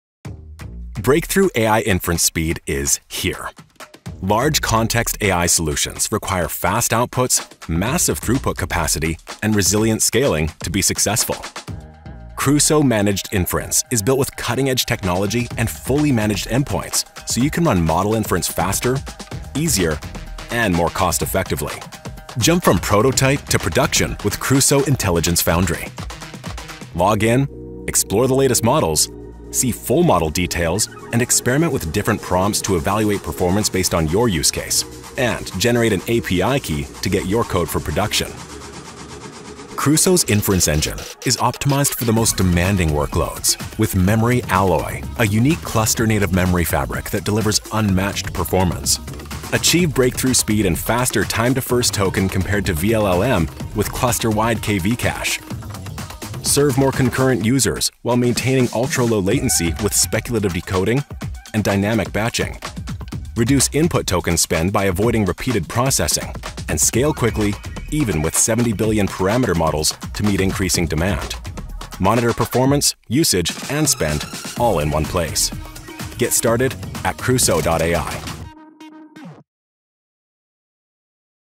Crusoe Managed Inference: Achieve 9.9x faster TTFT with Crusoe’s inference engine + MemoryAlloy tech
Ready to ship AI features faster than ever? The Crusoe Managed Inference service, powered by our breakthrough Crusoe inference engine, is here to give AI developers next-level speed and reliability.

See how Crusoe eliminates infrastructure complexity and drives unparalleled performance with its MemoryAlloy technology:

Breakthrough speed: Achieve up to 9.9x faster Time-to-First-Token* (TTFT): Our cluster-wide KV cache eliminates duplicate prefills, reducing complex 135K-token prompts from ~6 seconds to less than 1 second.

Superior throughput: Process up to 5x tokens per second* while reducing compute per token by up to 95% using speculative decoding.

Lower cost: Reduce input-token spend since each token is only processed once.

Seamless scaling: Meet changing workload demands with scaling that is managed for you, and reliable even when loading 70B parameter models.

Crusoe Intelligence Foundry: A simple, unified hub where you can explore and deploy top open-source models from providers like Llama, Gemma, Qwen, and DeepSeek. Experiment, generate API keys in seconds for the fastest path to production, and enable provisioned throughput to support your most intense traffic.

* Compared to vLLM for the Llama 3.3 70B model; see benchmarking methodology shared

➡️ Read the full launch details on the Crusoe Managed Inference blog